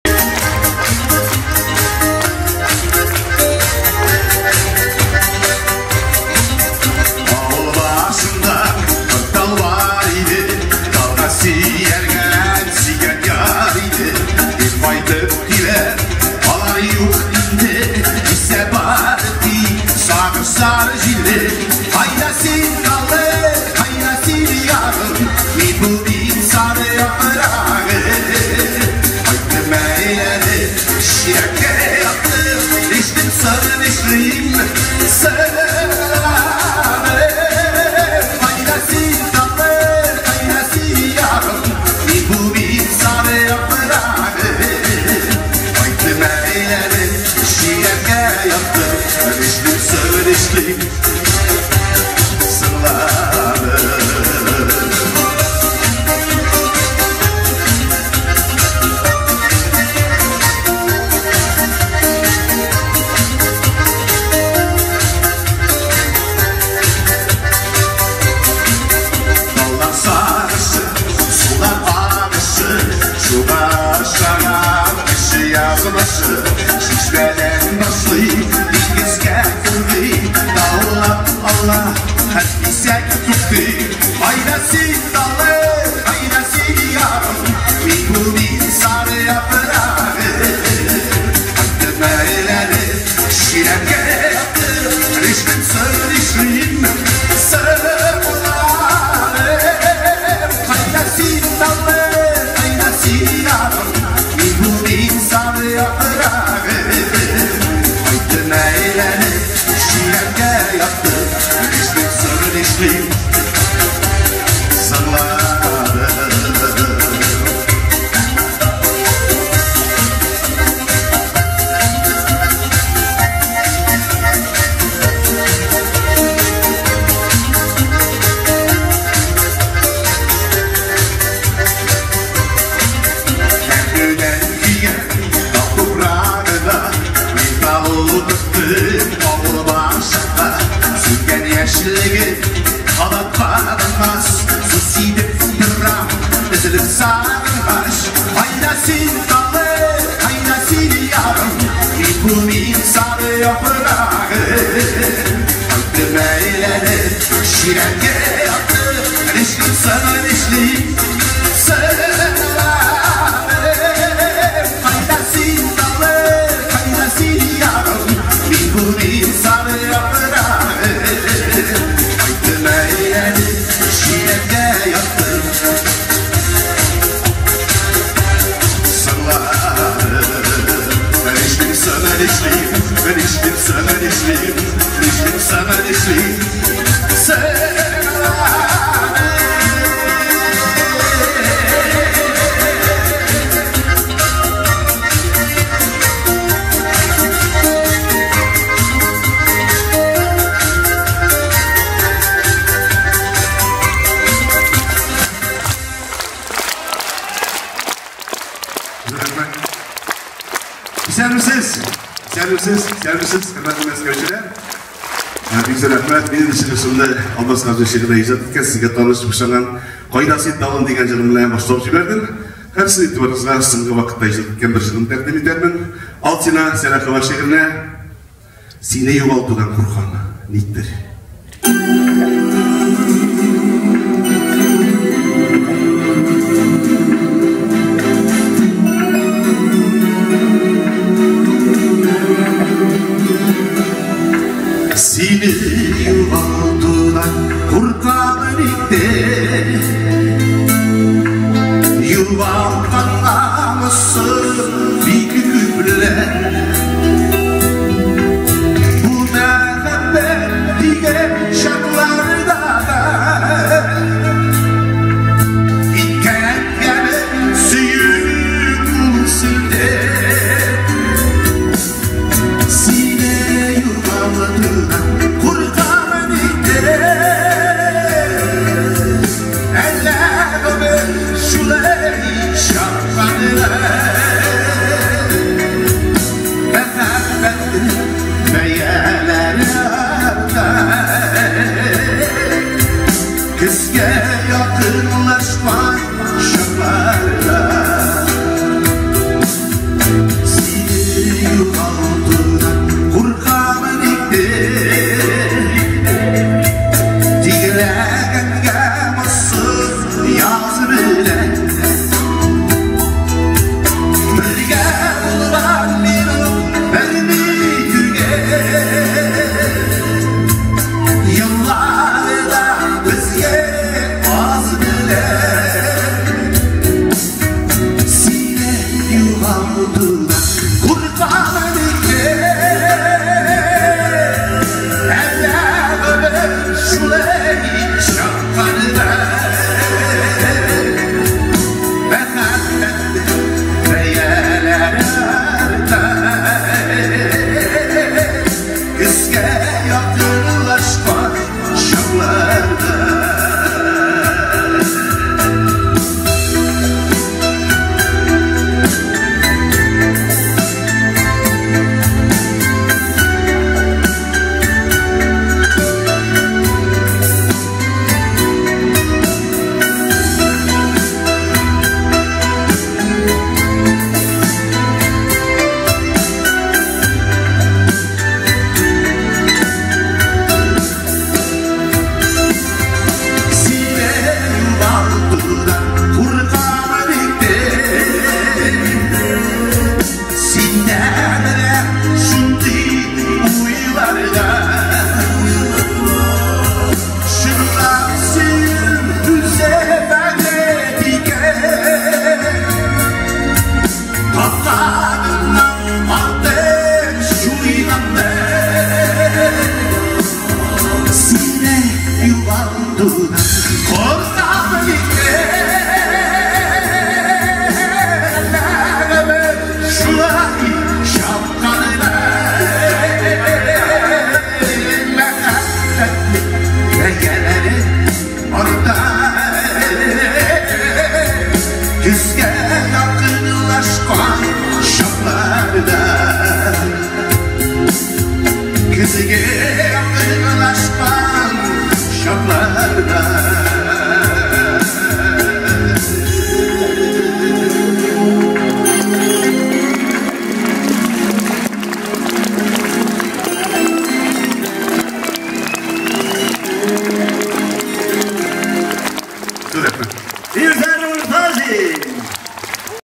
Фирзар Муртазин. Старые добрые песни
Салават Фатхетдинов и Фирзар Муртазин  дружат  и поют вместе уже много лет. Фирзар, частый гость на концерте Салавата.